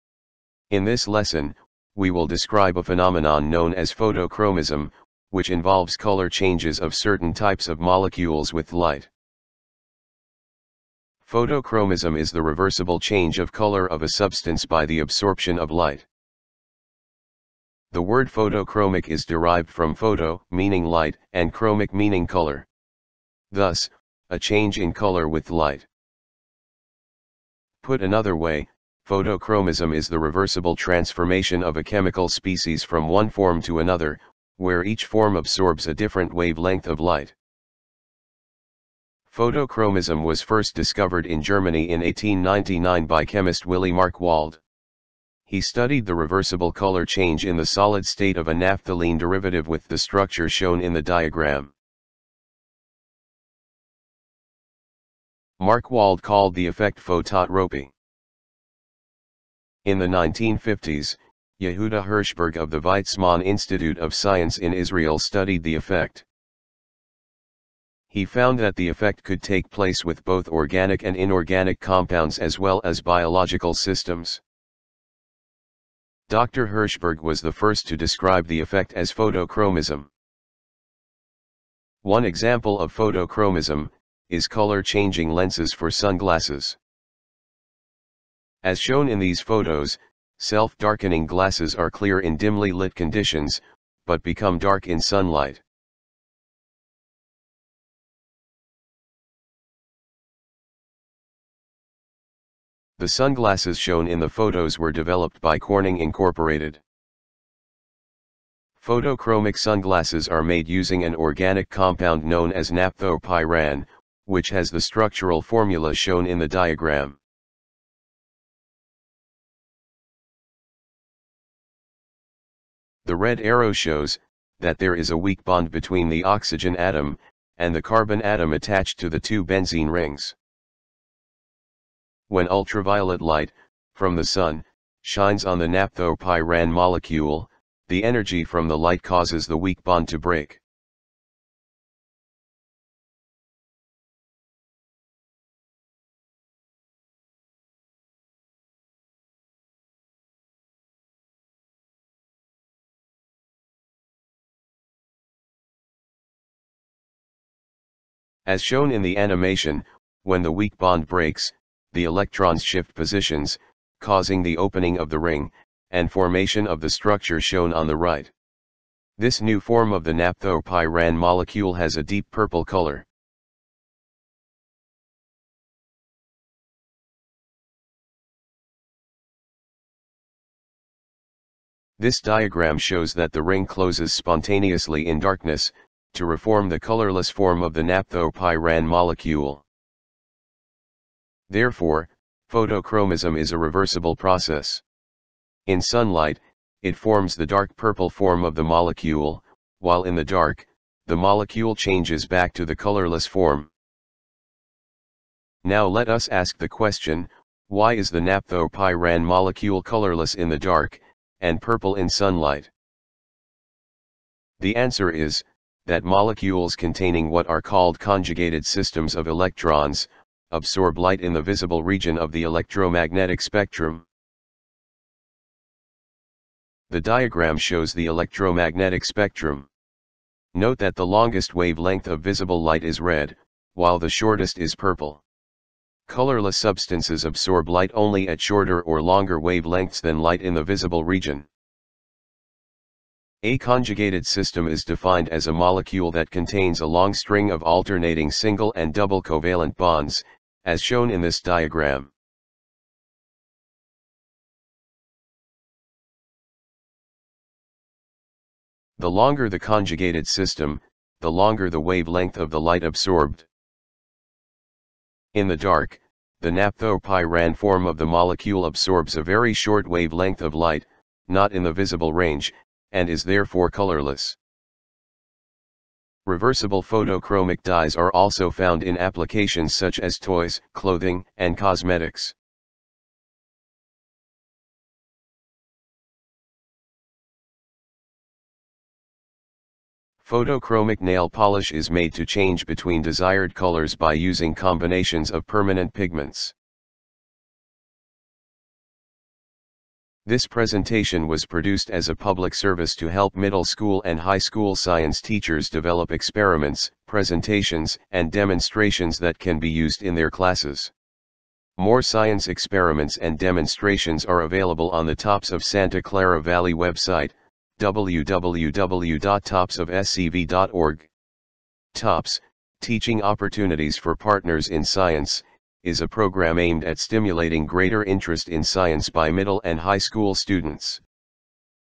Photochromism
This video lesson describes a phenomenon known as Photochromism, which  involves color changes of certain types of molecules with light.The word photochromic is derived from photo, meaning light, and chromic meaning  color.  Thus, a change in color with light. Photochromism is the reversible change of color of a substance by the absorption of light. Presented by TOPS of Santa Clara Valley for STEM instruction.  See topsofscv.org.